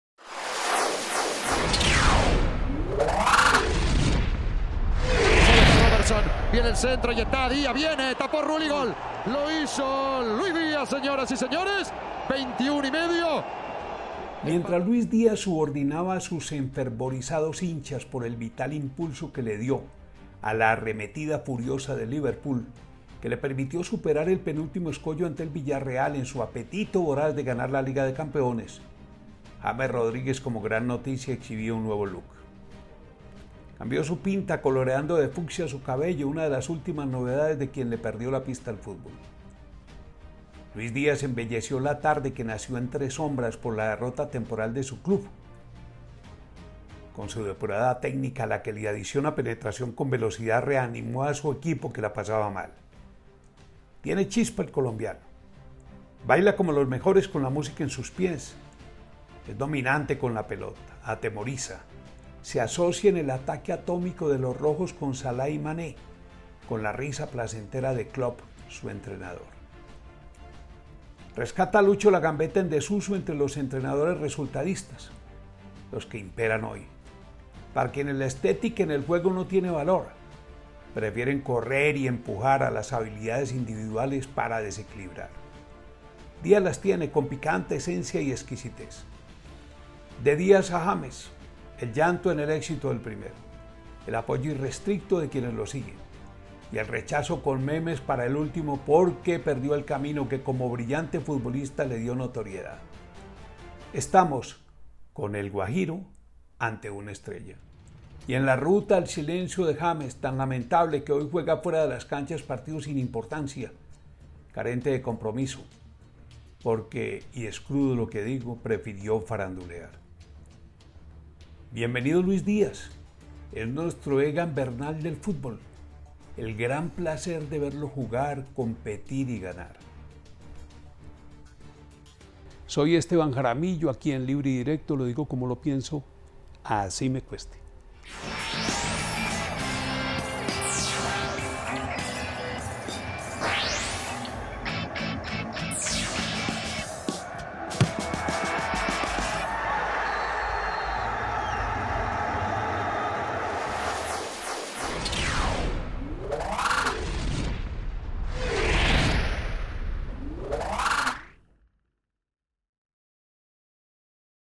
LUIS DÍAZ LA FURIA DEL ATAQUE EN EL LIVERPOOL
Un indígena bailarín. El placer de verlo, jugar, competir y ganar, mientras James se pierde en los laberintos de la farándula. Luis Díaz, nuestro Egan Bernal del futbol

ESTEBAN JARAMILLO